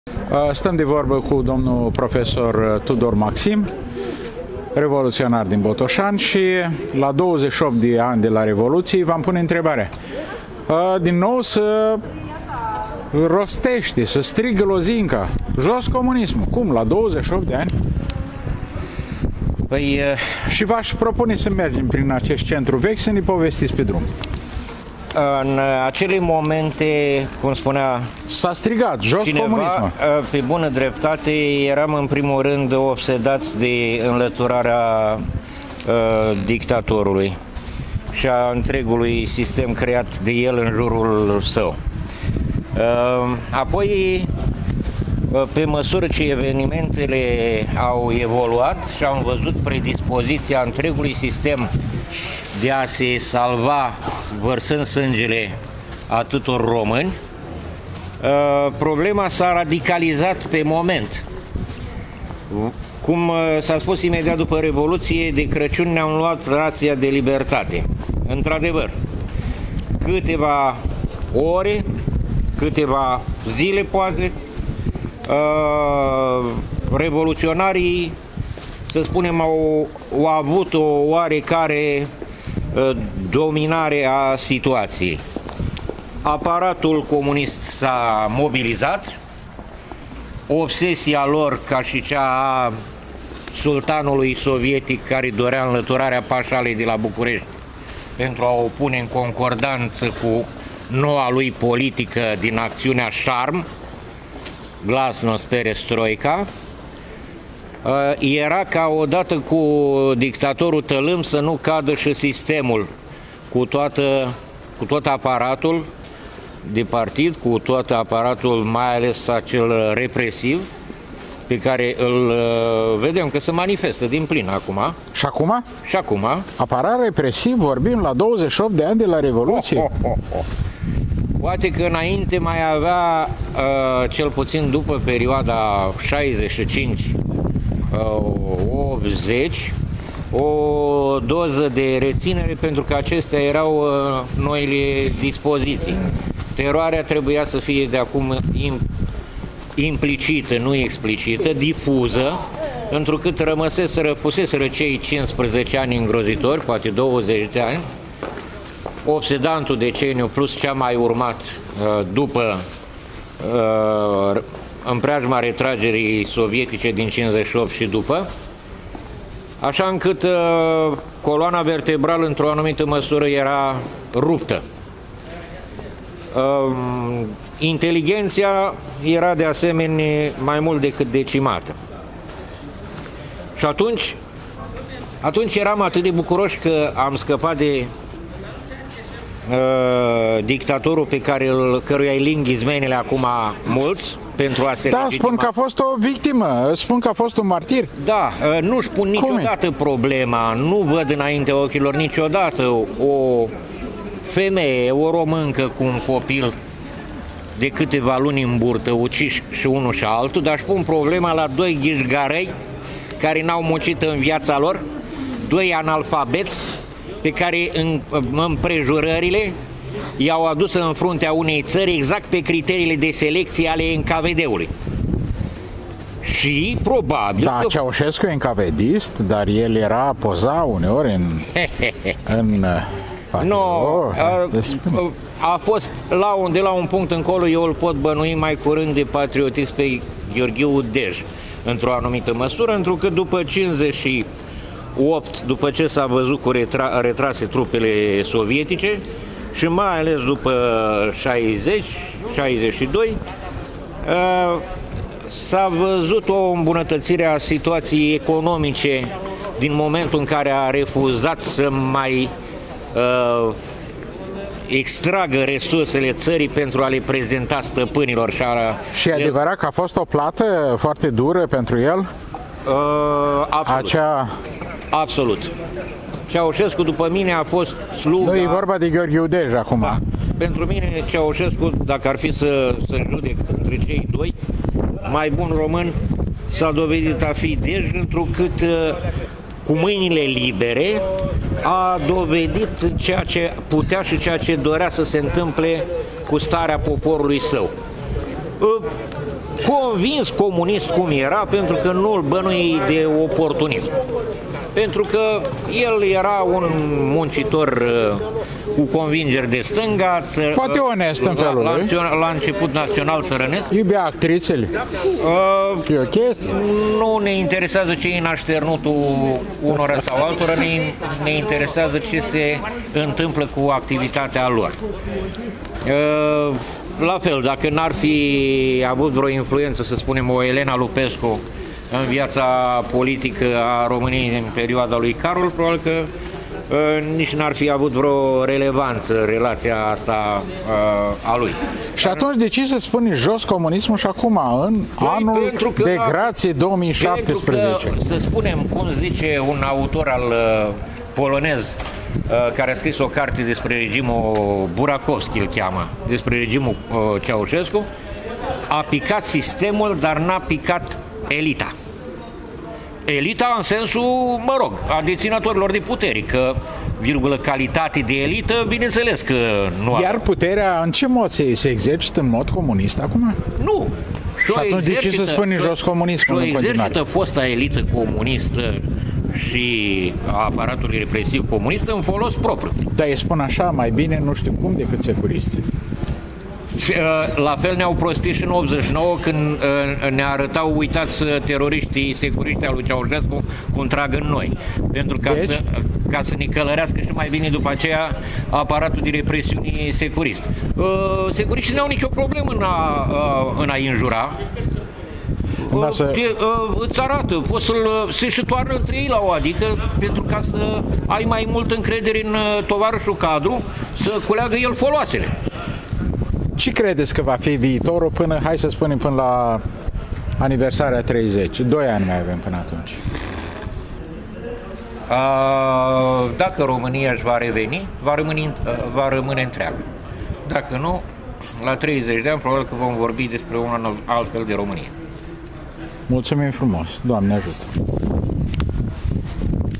Prof Tudor Maxim despre Revoluția română
Revoluția din 22 decembrie 1989 la Botoșani.
Comemorare 22 decembrie 2017